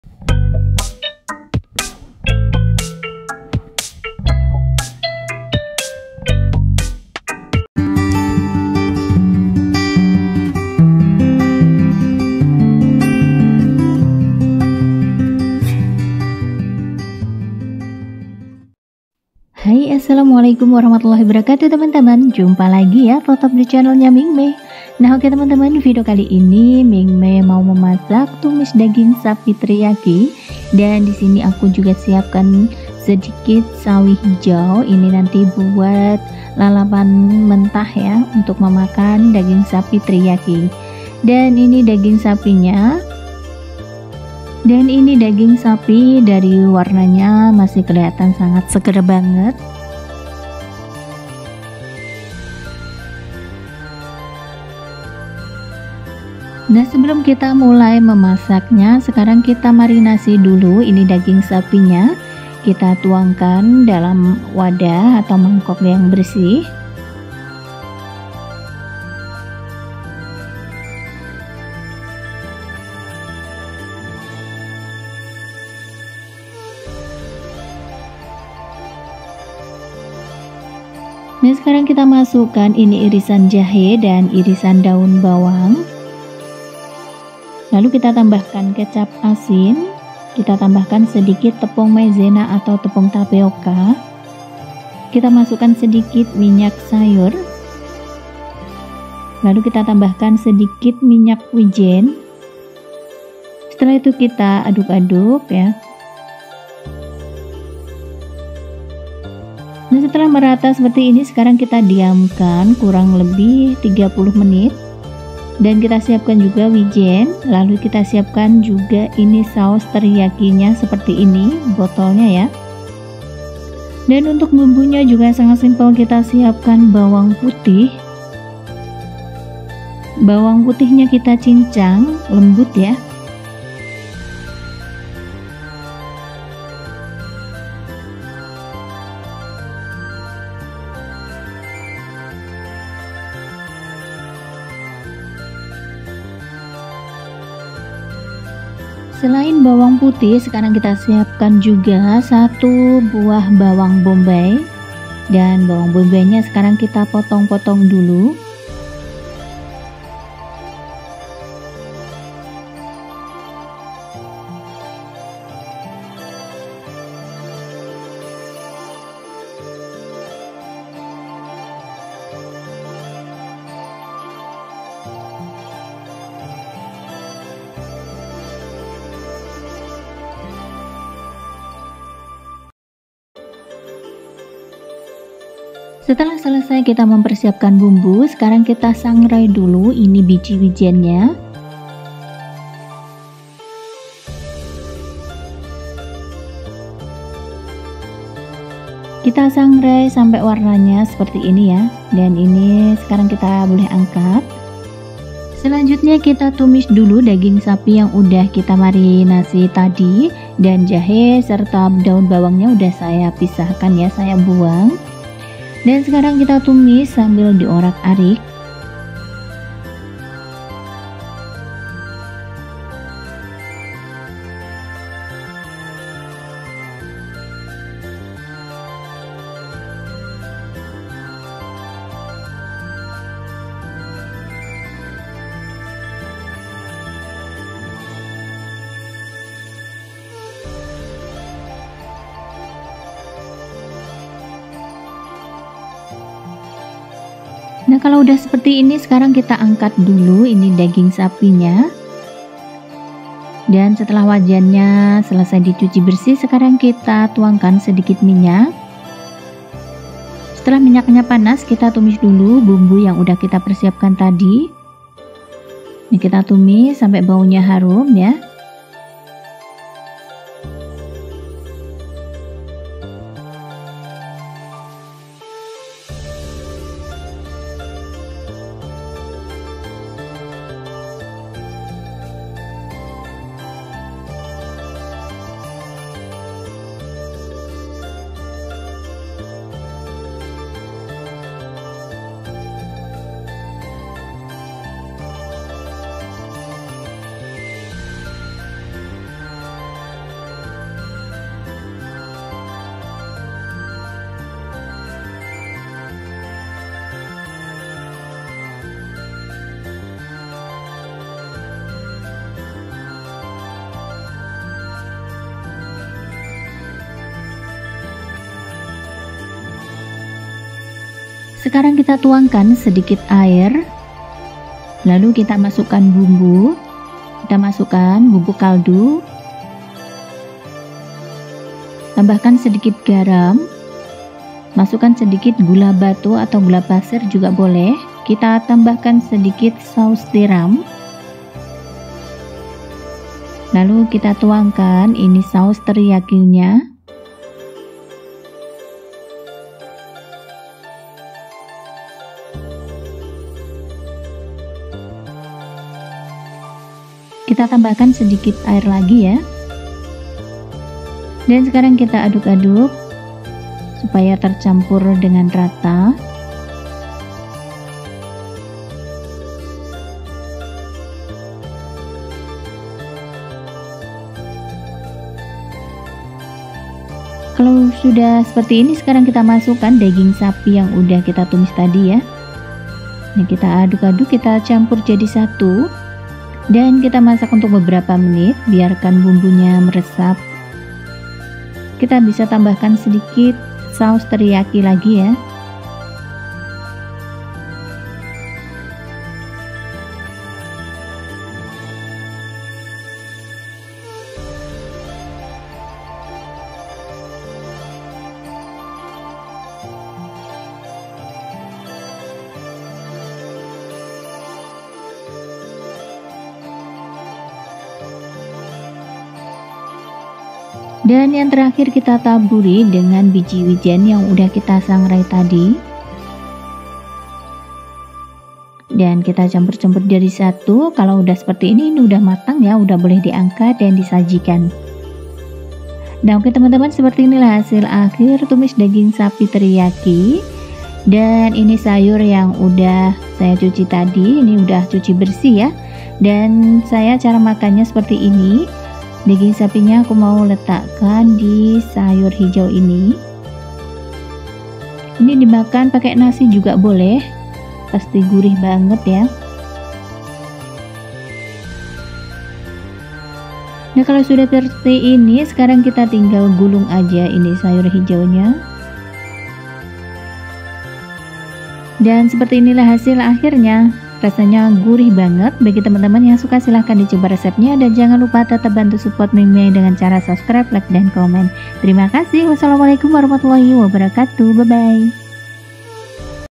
resep masak beef teriyaki ala resto || daging sapi teriyaki //masakan taiwan sehari hari
assalamualaikum wr wb kawan di video kali ini aku mau masak tumis daging sapi teriyaki
cara masak nya silahkan simak video saya sampai selesai di jamin mudah untuk di pelajari dan jangan lupa tetep bantu support terus channel MINGMEI dengan cara

bahan; daging sapi
              sawi selada
bumbu marinasi:
               kecap asin
               jahe ,daun bawang
               minyak wijen hitam
               minyak goreng,tapioka
bumbu tumis;
               bawang putih cincang
               bawang bombay
               bubuk kaldu
               garam,gula
               saus tiram,biji wijen
               saus teriyaki